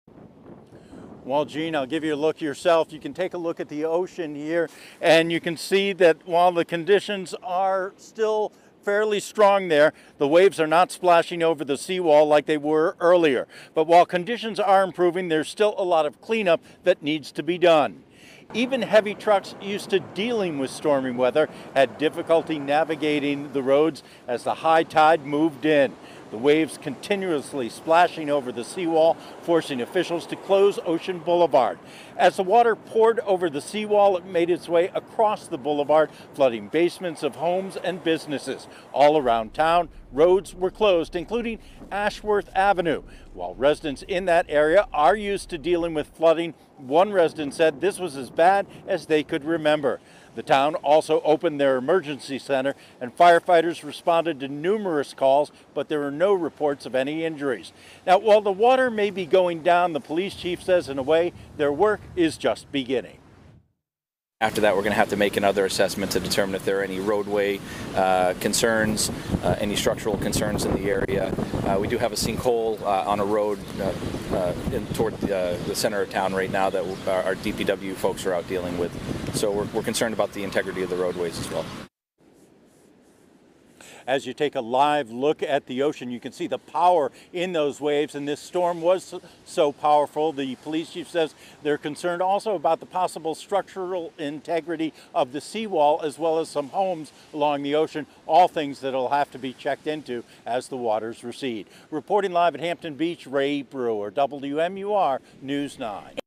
Hampton opens emergency center amid morning flooding
High tide at Hampton Beach was at 9:50 a.m., but flooding and splash-over continued as the tide receded and the strong winter storm continued to pull away.